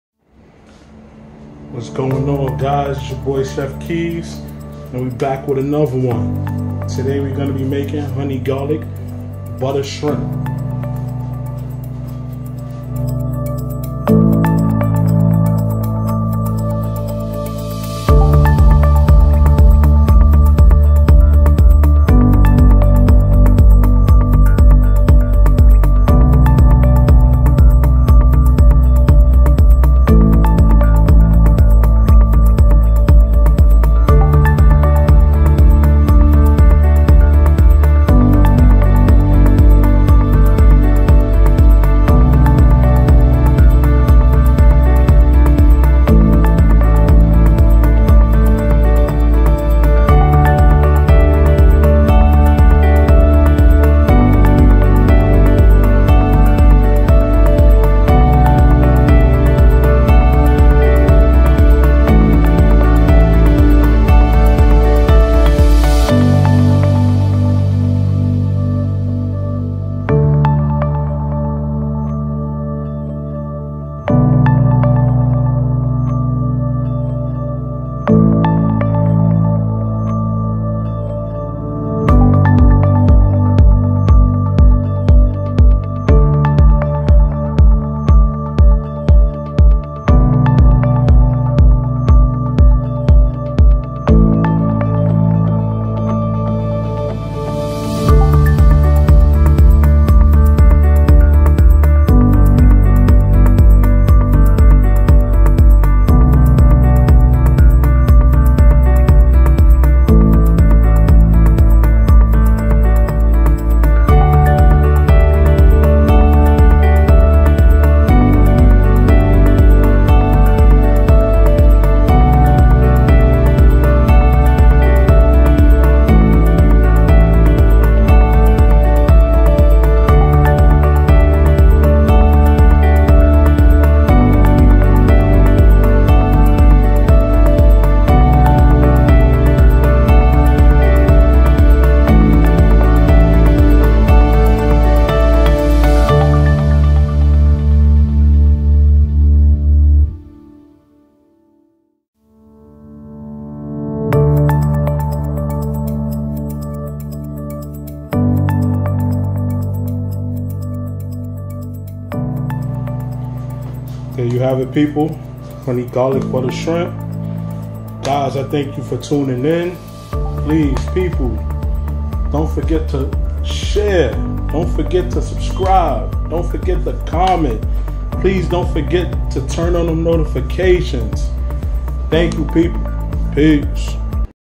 How To Make Honey Garlic Butter Shrimp
Welcome back to another video. This video will show you how you can make honey garlic butter shrimp. You can use any size shrimp you like as well.